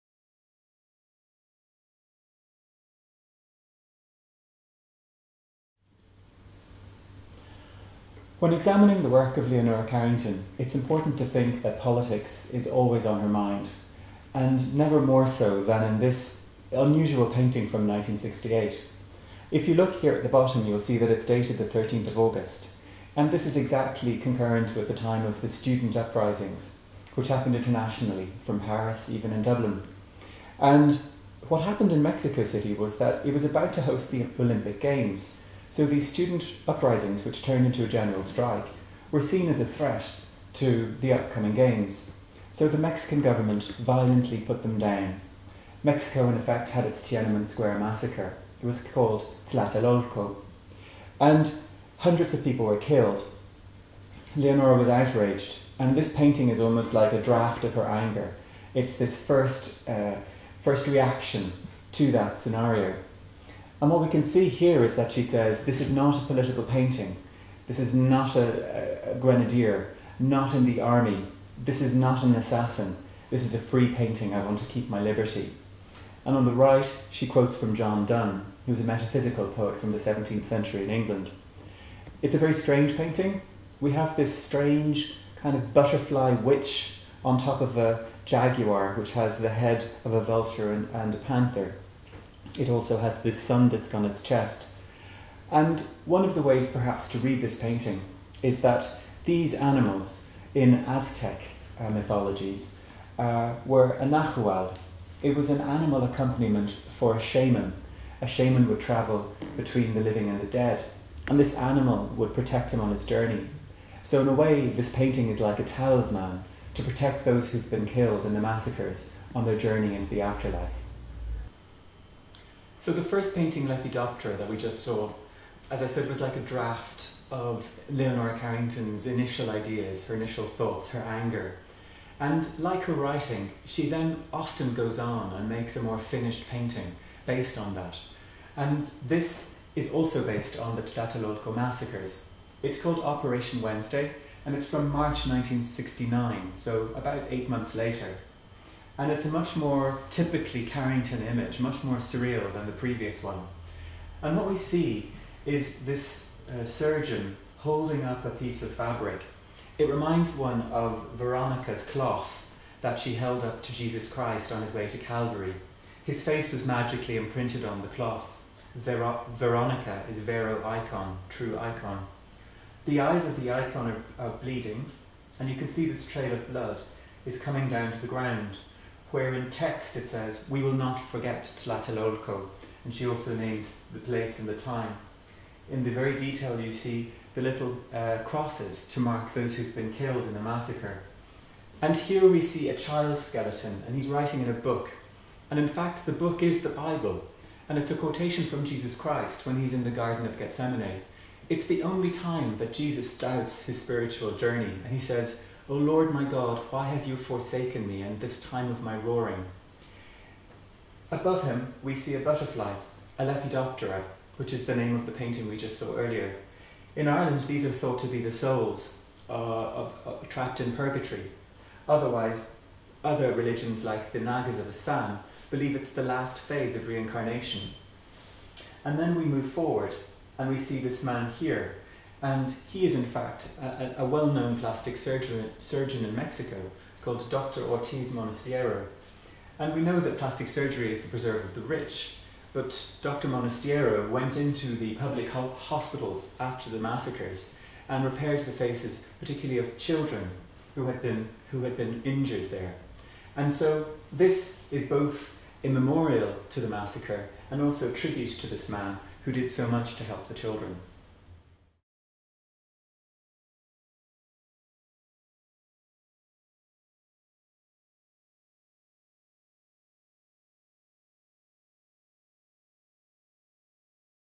Leonora Carrington & Politics IMMA
Curator Seán Kissane discusses two paintings by artist Leonora Carrington and their reference to political events.  Leonora Carrington: The Celtic Surrealist, Irish Museum of Modern Art